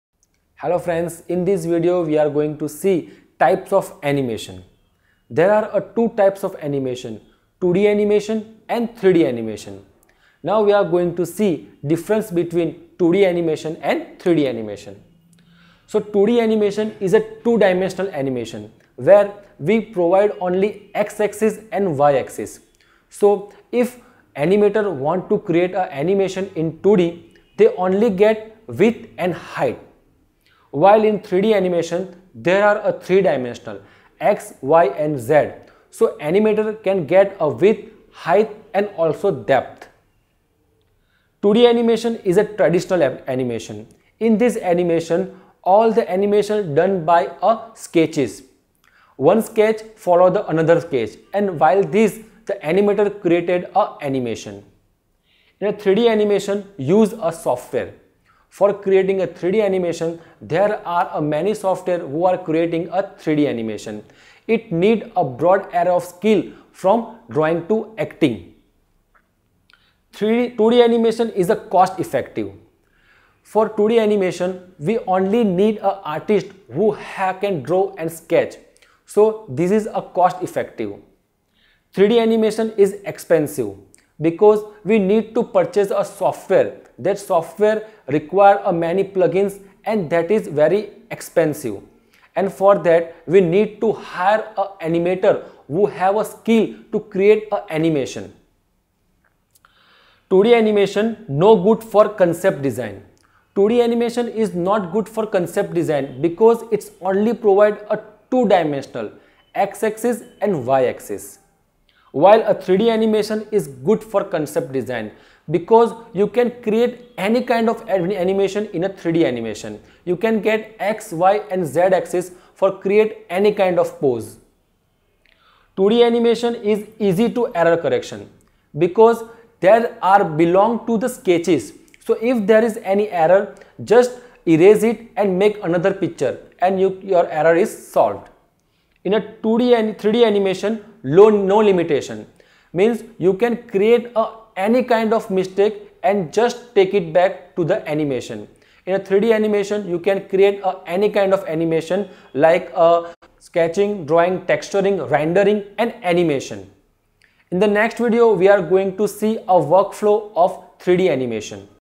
3D Animation - Types of Animation
3D Animation - Types of Animation

Lecture By: Mr. Rushi Panchal, Tutorials Point India Private Limited.